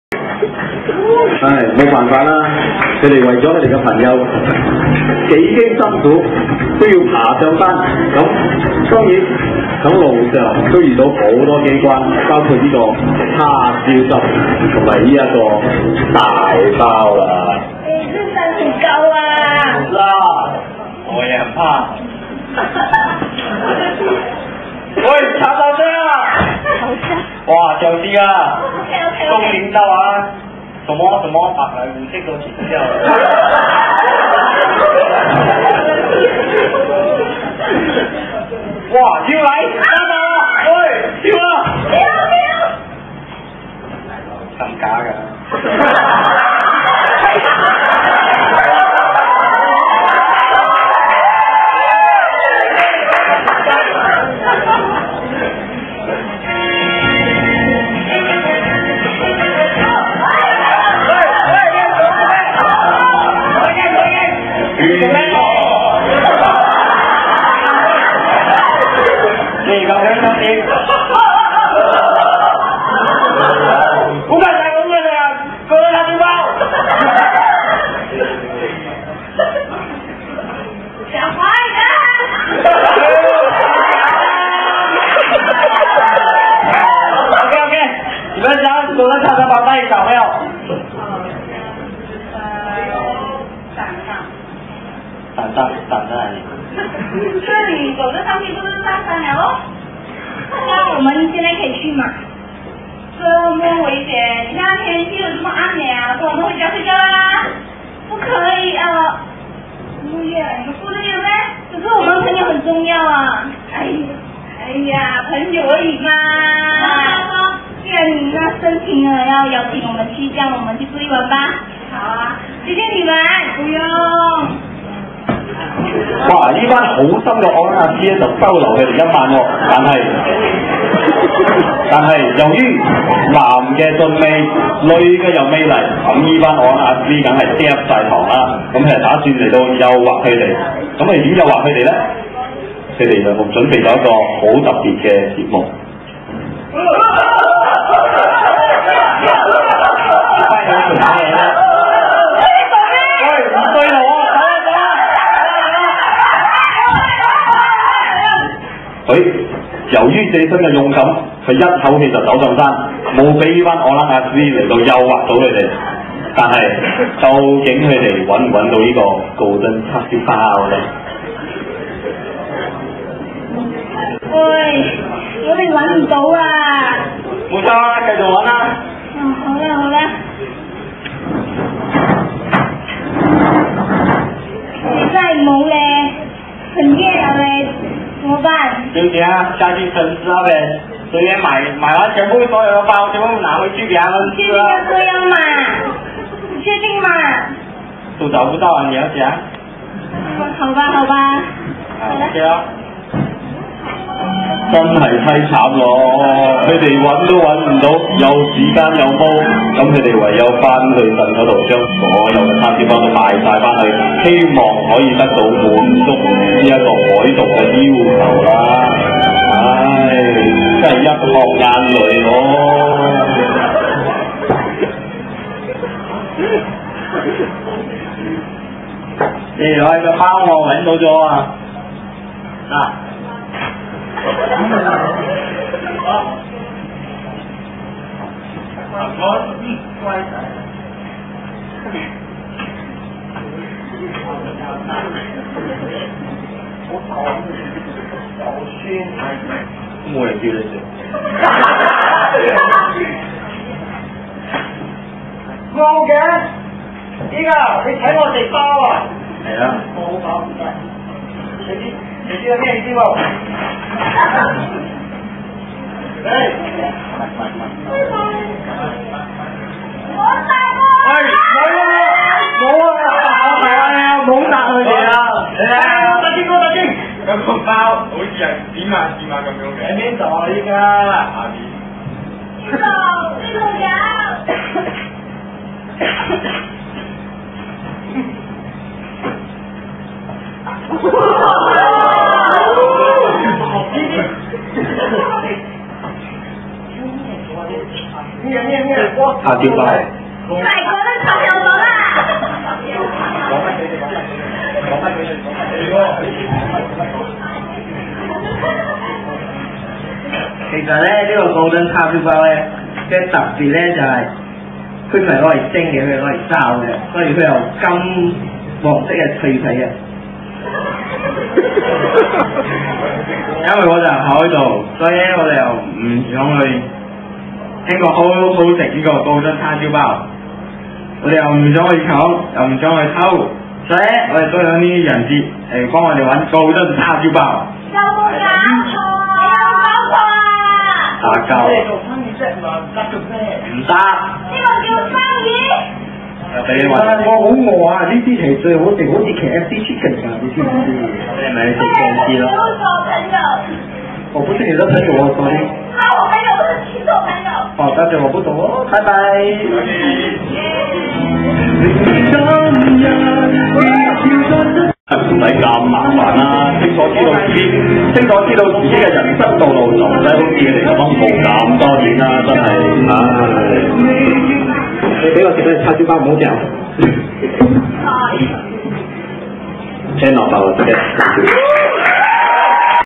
No Apologies Camp 2008 Talent Nite - Forever (Adventure) (Part 2)
Rules: Participants were given a CD soundtrack containing 6 random songs, and a movie genre to act out. Forever chose Adventure.

Starring Chi Yuan, Tiffany, Peter, Harry, Austin, Wei Wei, Jason, Steffi, Jerryni, Weng Hung, Kean Ming and many more!

Synopsis: 2 couples are captured by a group of pirates during their vacation. In exchange for their freedom, the pirate leader orders to a dangerous expedition in search of the... wait for it... Golden Char Siew Pau.